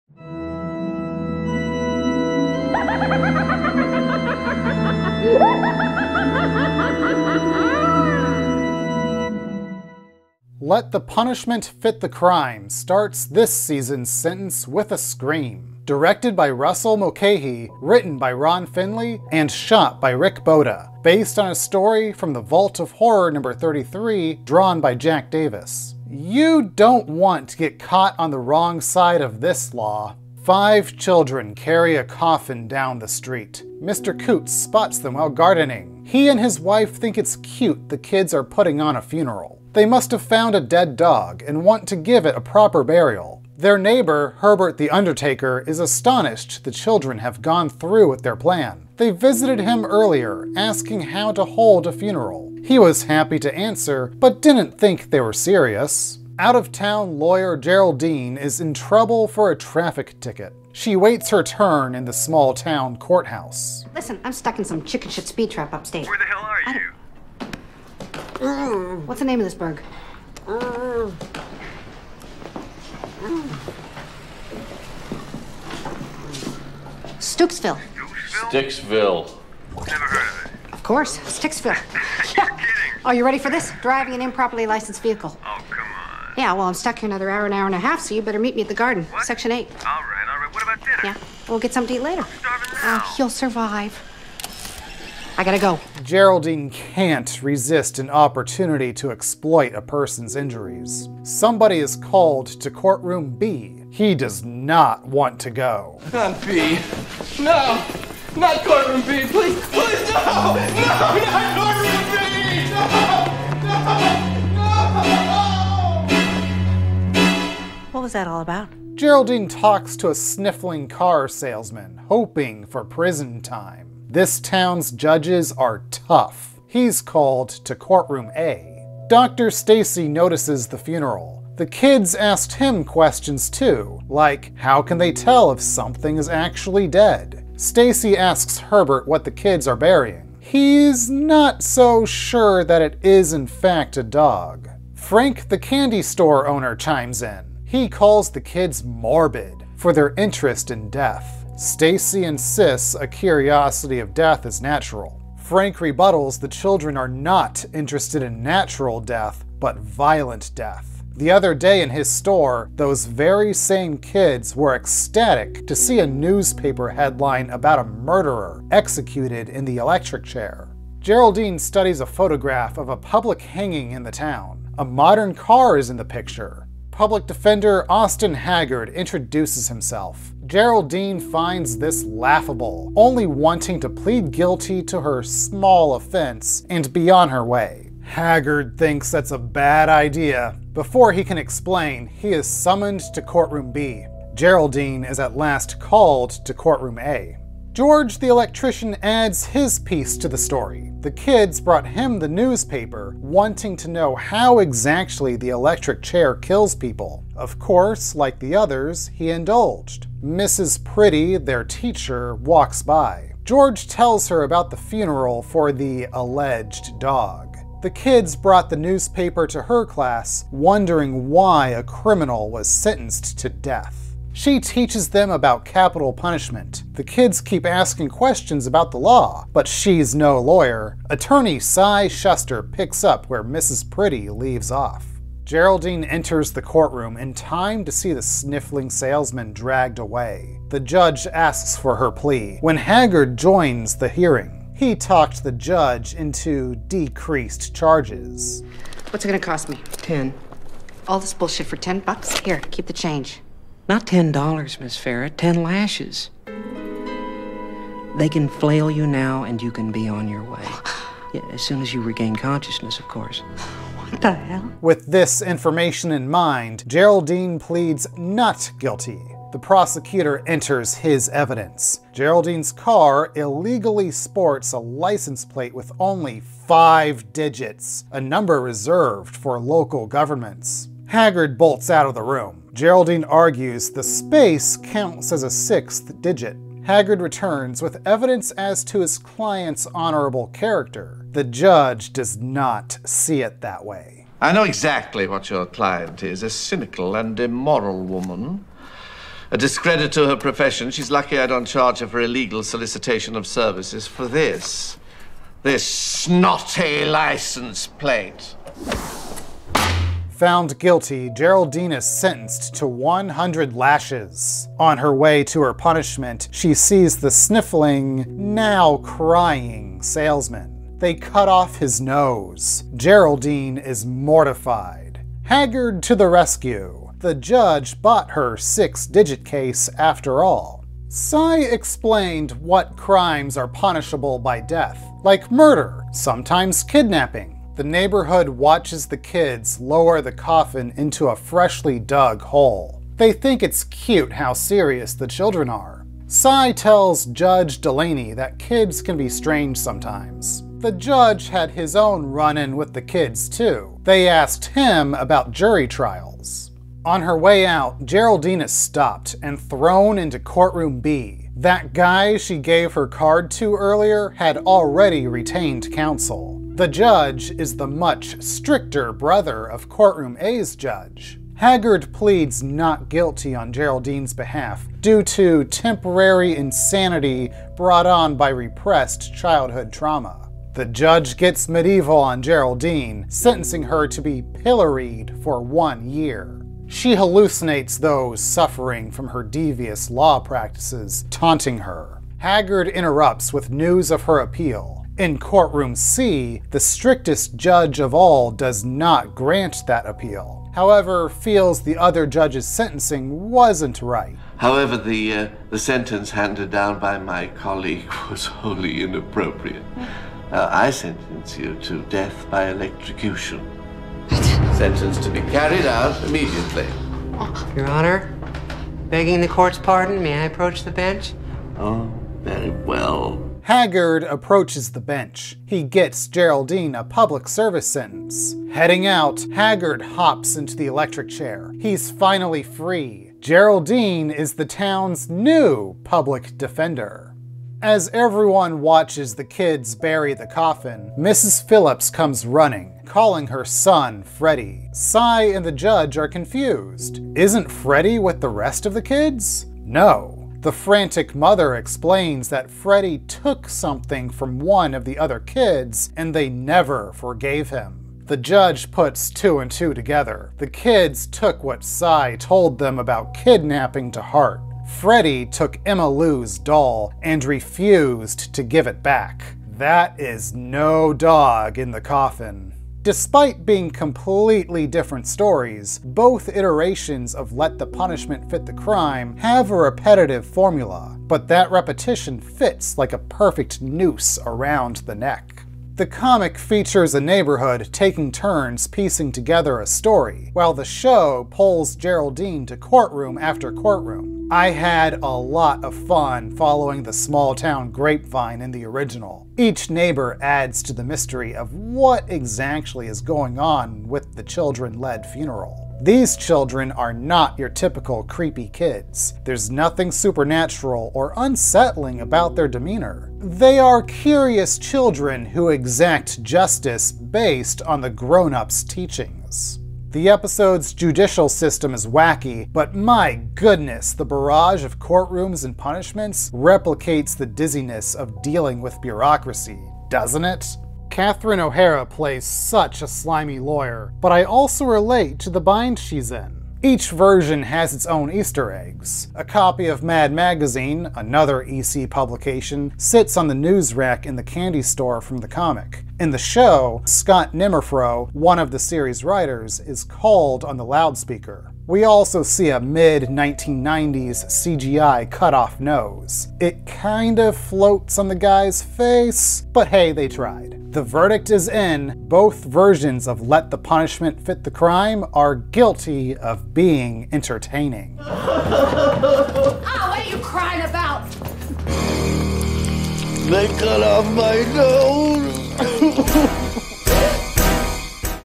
Crypt Keeping: Season 6, Episode 1 - Let the Punishment Fit the Crime
Happy Judgment Day! In this week's case, Catherine O'Hara plays a lawyer on the wrong side of the law, while in the comic a group of creepy kids exact some justice of their own. All is fair in the court when you "Let the Punishment Fit the Crime!"

Tell me what you think about Tales from the Crypt and Let the Punishment Fit the Crime in the comments!